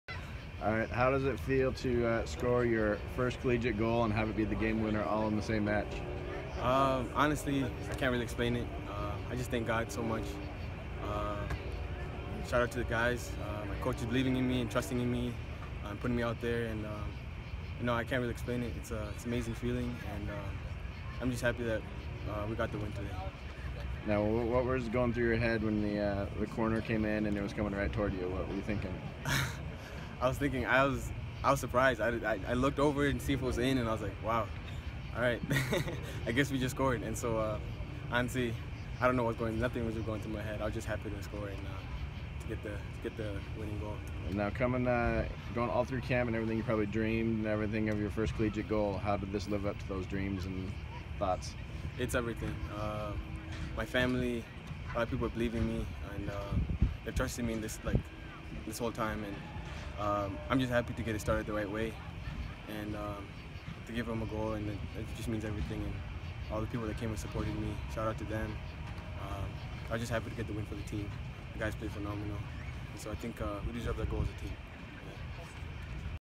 MSU Men’s Soccer Post-Game vs USF - Farai Mutatu
Spartan freshman forward Farai Mutatu talks to the media after he netted the game-winning goal in his Spartan debut as No. 7 MSU beat South Florida, 1-0, on Friday, Aug. 24 at DeMartin Stadium. With the 1-0 victory for MSU, the Spartans won their home opener for the sixth-straight season.